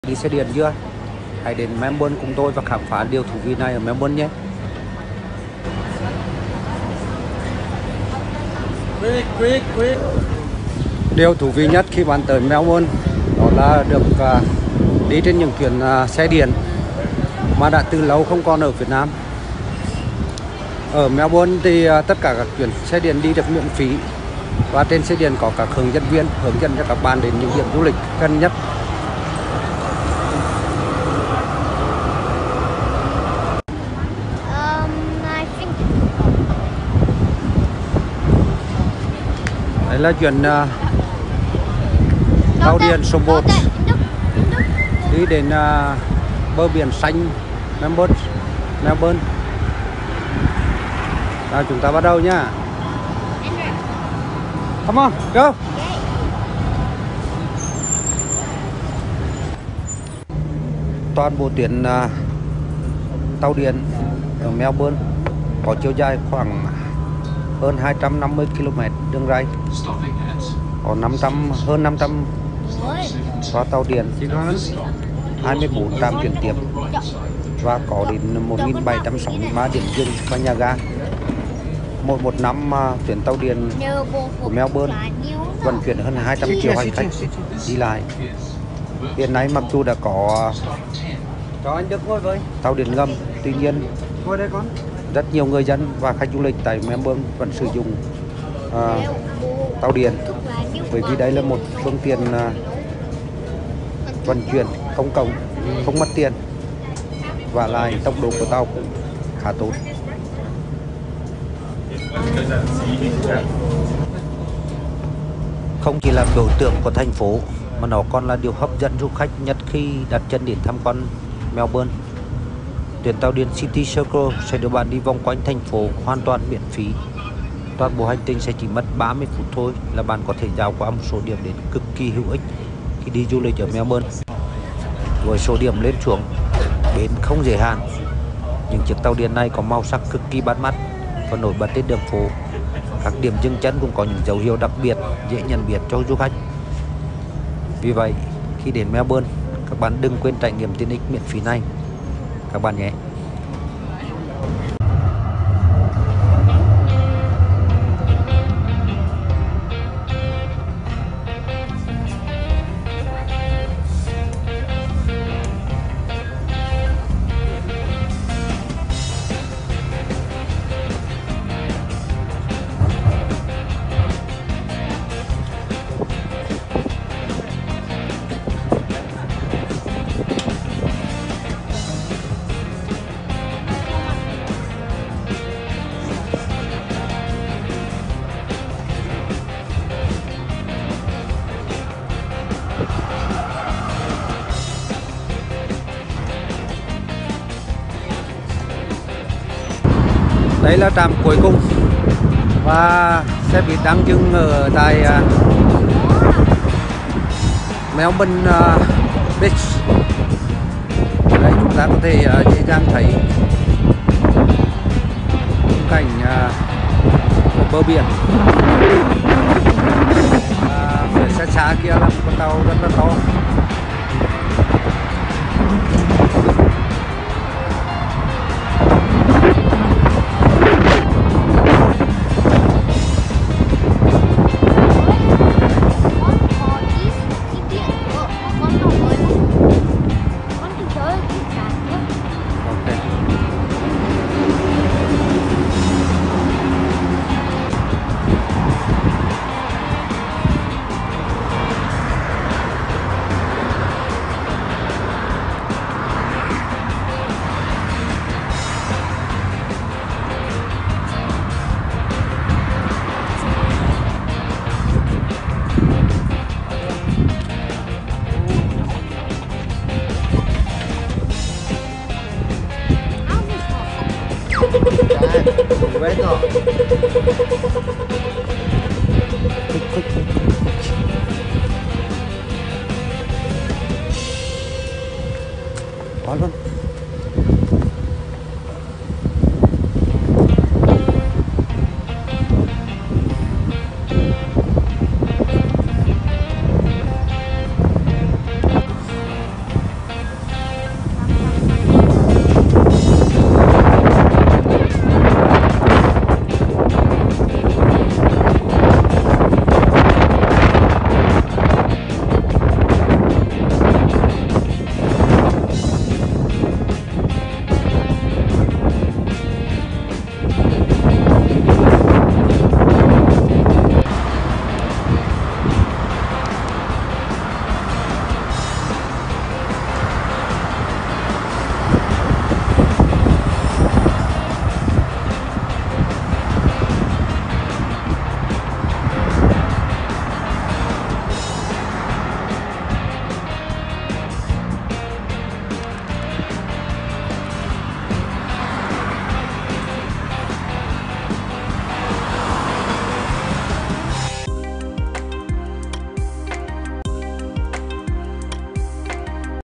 Trải nghiệm xe điện tại Melbourne với anh em nhà Tèo
The city's Free Tram Zone extends from Queen Victoria Market to Docklands, Spring Street, Flinders Street Station and Federation Square. ... Hop on and off the historic trams as often as you like. You don't need a myki if you're only travelling in the city's Free Tram Zone or on the City Circle Tram.